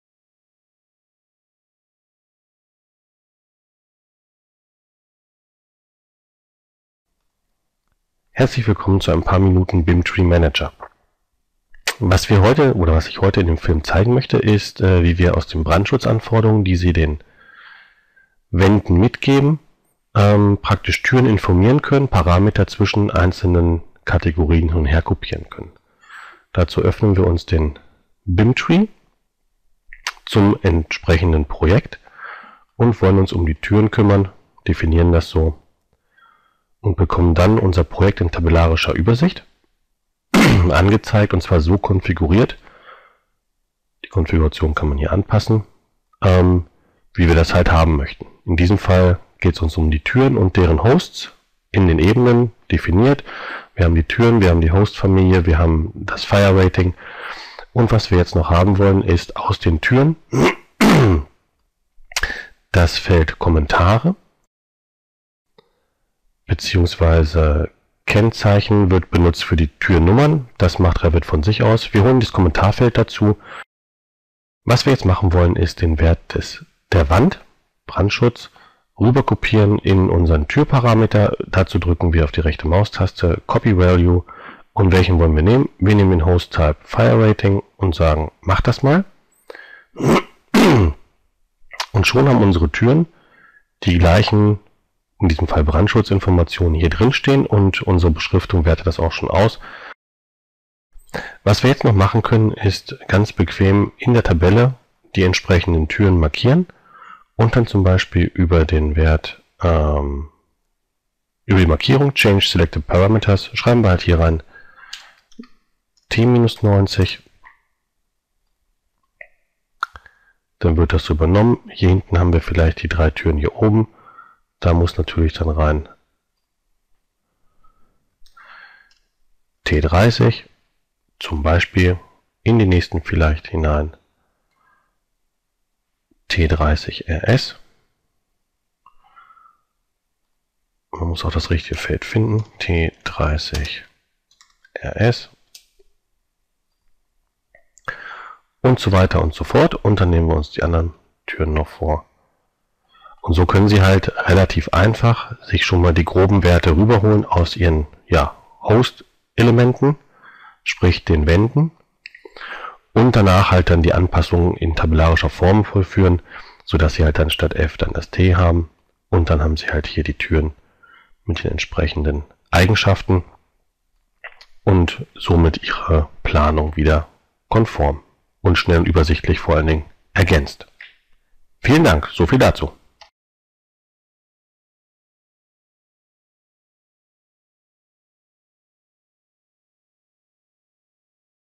BIM Tree Manager
Beispiel: schnelles herausfiltern aller Türen, die in Wänden mit ausgefüllten Brandschutzanforderungen eingeplant sind / die Feuerwiderstandswerte von der Wand in die Tür kopieren / tabellarisches Bearbeiten und Ändern dieser Werte...